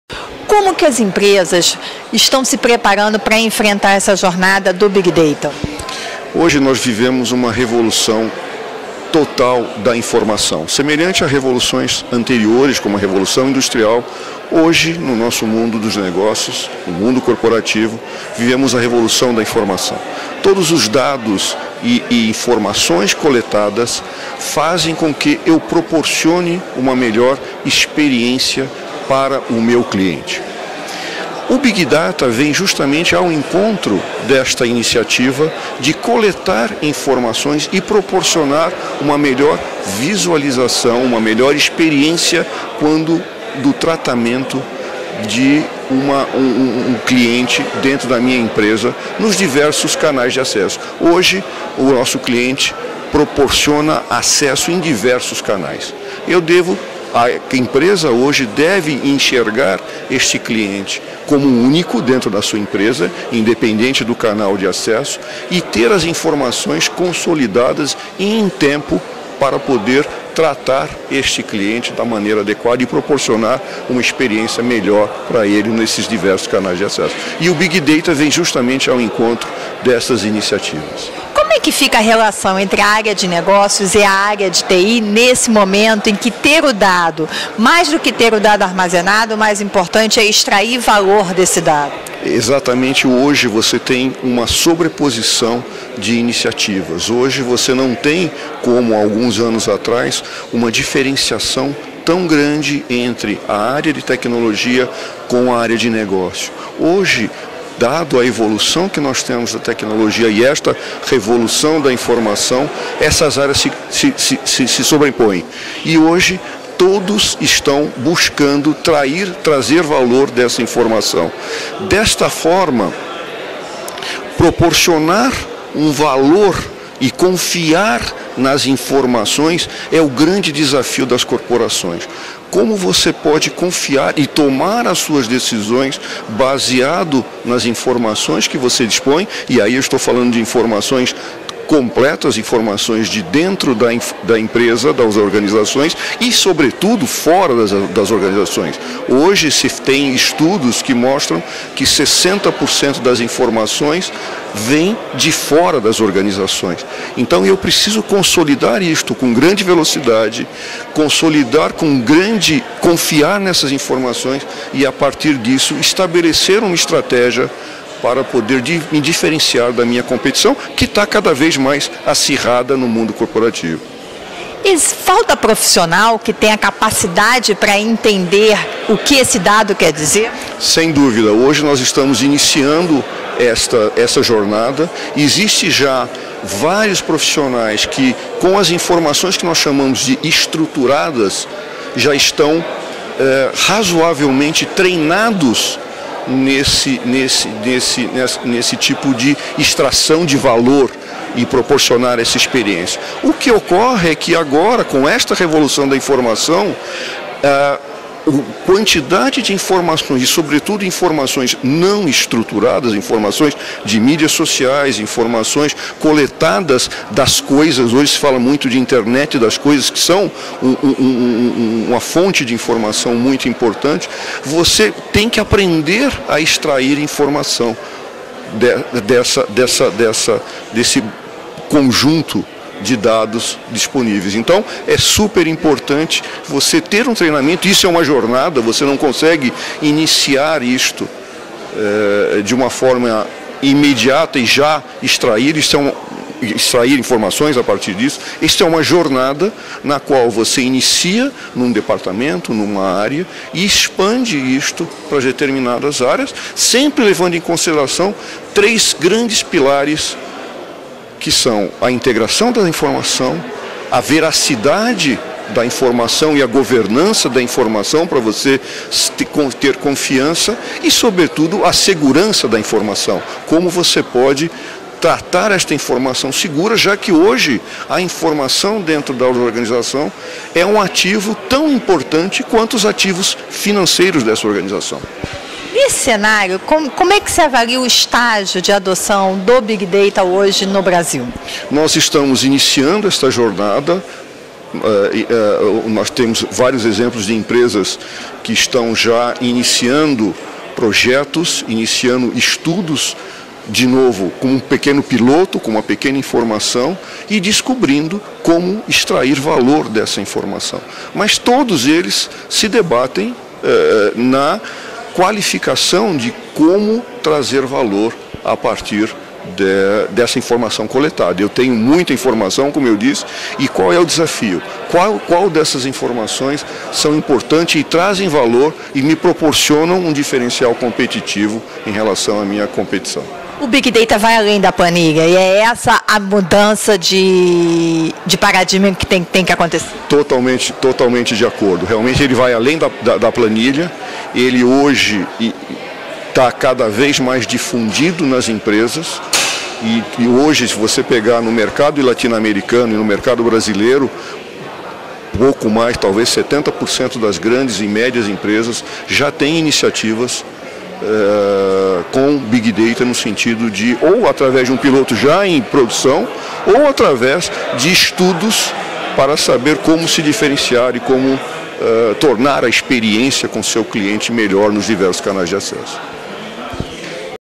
Confiar na informação do big data é o desafio da gestão corporativa
O mundo corporativo vive o momento de uma revolução da informação e é essa transformação que impulsiona os projetos de big data. "Hoje há vários canais de atendimento e o consumidor quer ter o mesmo atendimento em qualquer um deles. O big data surge para consolidar os dados para gerar a informação diferenciada", explica o lider de MDM da Informatica para a América Latina, Francisco Casella, em entrevista à CDTV, durante o Big Data Summit, realizado no dia 08/03, em São Paulo.

De acordo com o executivo, proporcionar valor e confiar na informação gerada pelo dado para tomar a melhor decisão é, hoje, o maior desafio das organizações, uma vez que a maior desses dados está vindo de fora dos muros e do controle direto dos gestores de TI e dos diretores das empresas. "A maior parte dos dados vem de forma não-estruturada, coletadas nas mídias sociais e, amanhã, dos objetos conectados pela Internet das Coisas", destaca o diretor da Informatica.

Segundo ainda Casella, o big data, hoje, funciona baseado em três grandes pilares: a integração, a veracidade e a governança e a segurança da Informação. "A informação é hoje um ativo tão relevante quanto os financeiros", adverte. Assistam a entrevista.